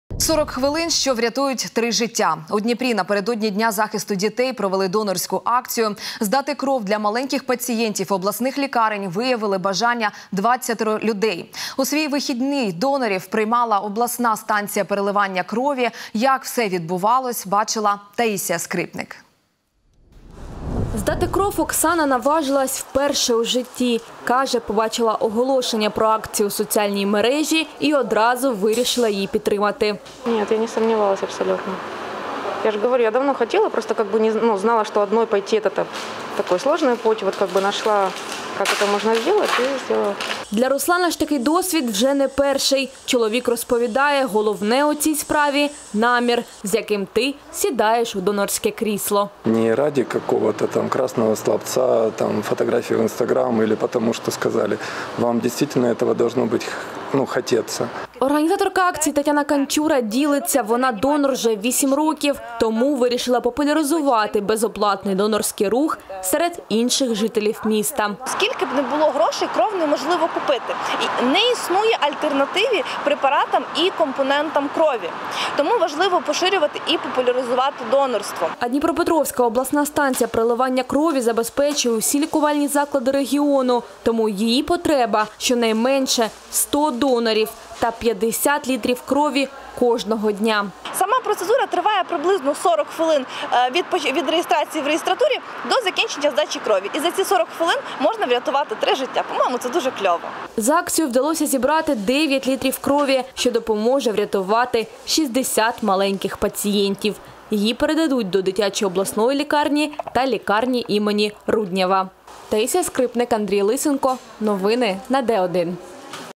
У Дніпрі до Дня захисту дітей провели донорську акцію
40 хвилин, що врятують три життя. У Дніпрі напередодні дня захисту дітей провели донорську акцію. Здати кров для маленьких пацієнтів обласних лікарень виявили бажання двадцятеро людей. У свій вихідний донорів приймала обласна станція переливання крові. Як все відбувалось бачила Таїсія Скрипник.